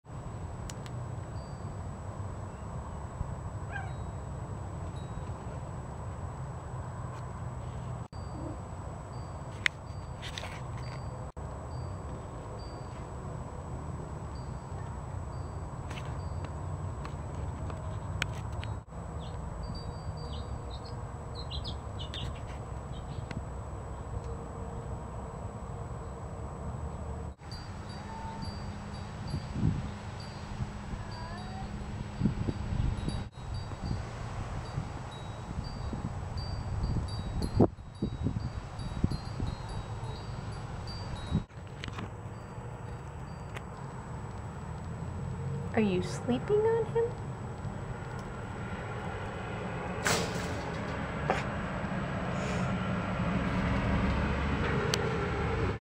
Bonding Bengals
Mia the new bengal kitten spends some time outdoors with her new bengal brother.  They are really learning how to get along well, it only took about a week of settling in.